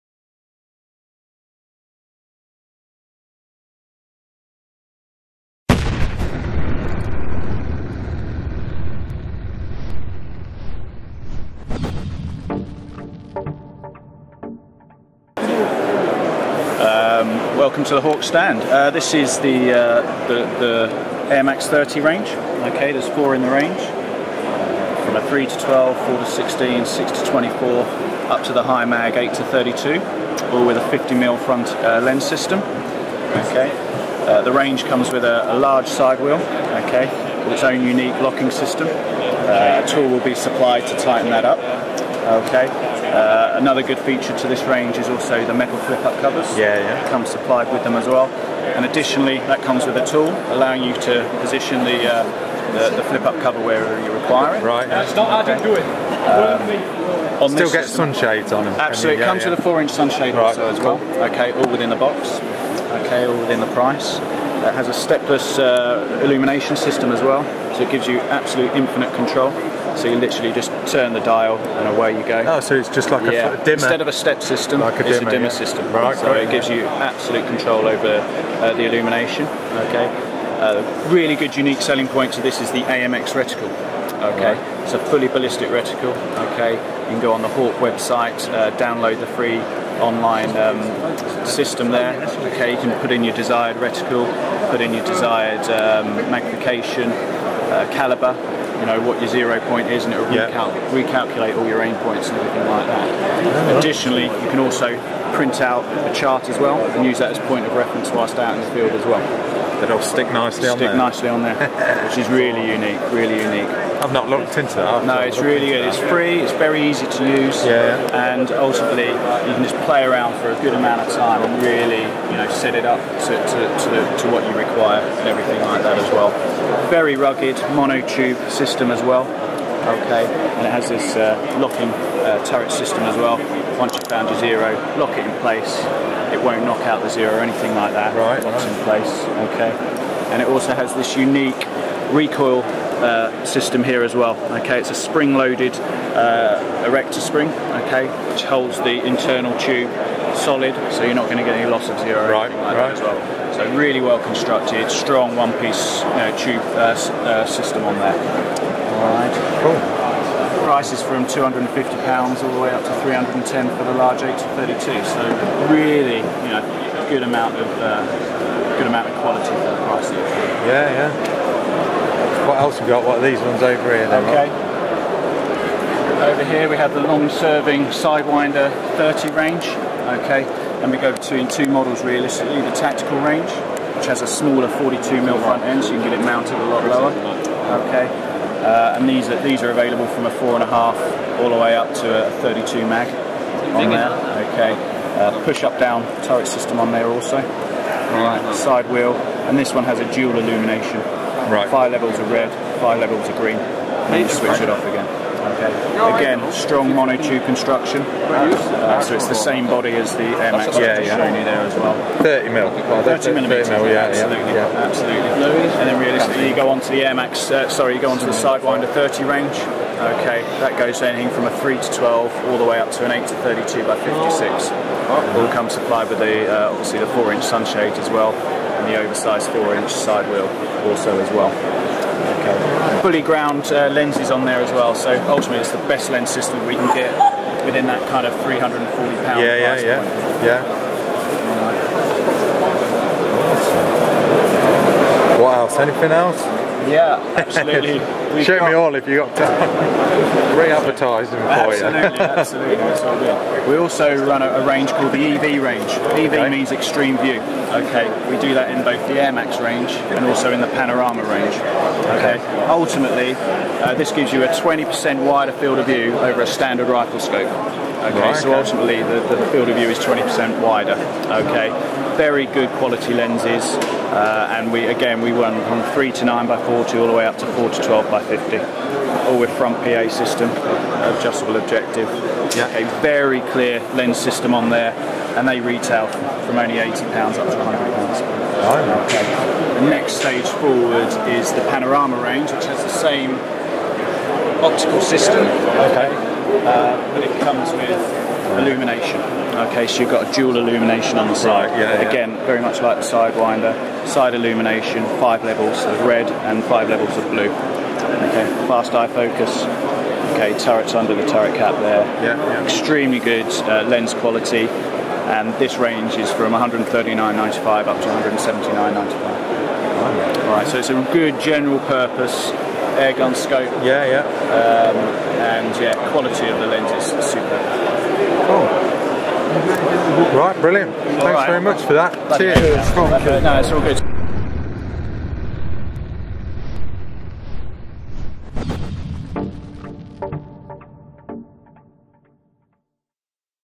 Hawke Scopes (UK Shooting Show 2015) by RACKNLOAD
Like the channel? Hawke Scopes booth review at the British Shooting Show 2015